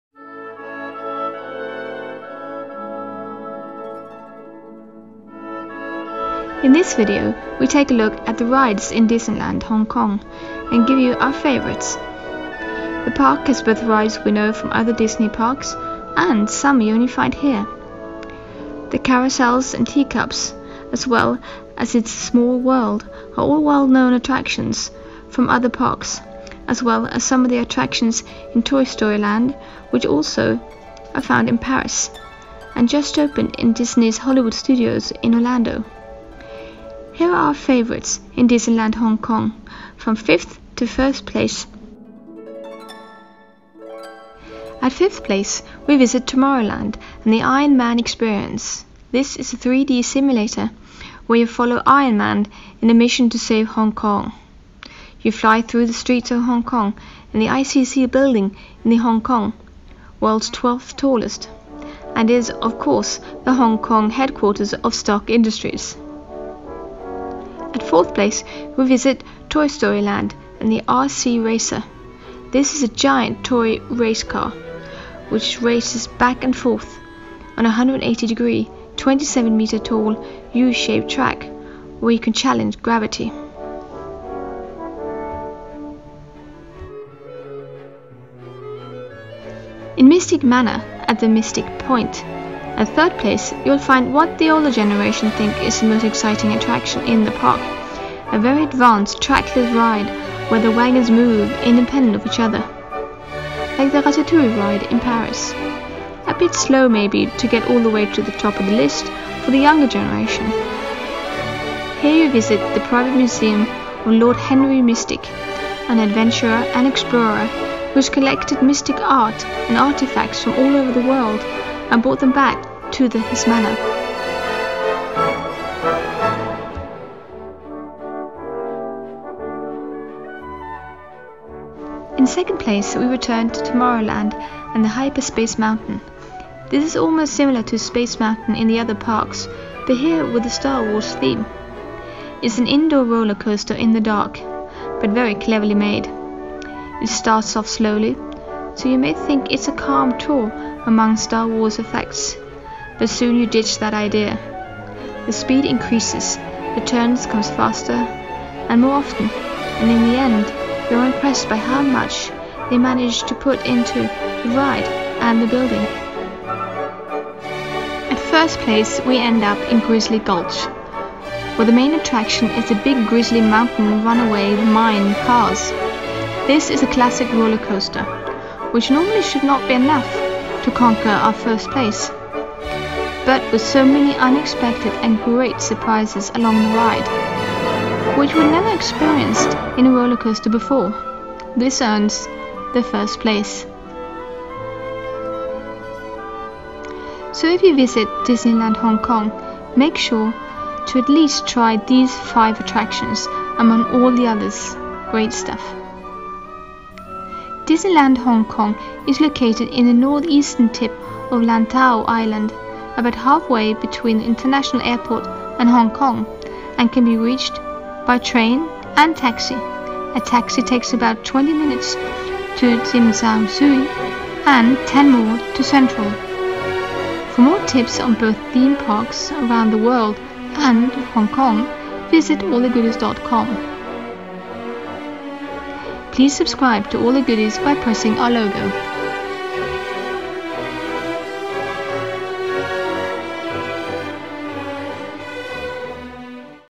The best rides in Disneyland Hong Kong | allthegoodies.com
In this video we take a look at the rides in Disneyland Hong Kong and give you our favorites. The park has both rides we know from other Disney Parks and some you only find here.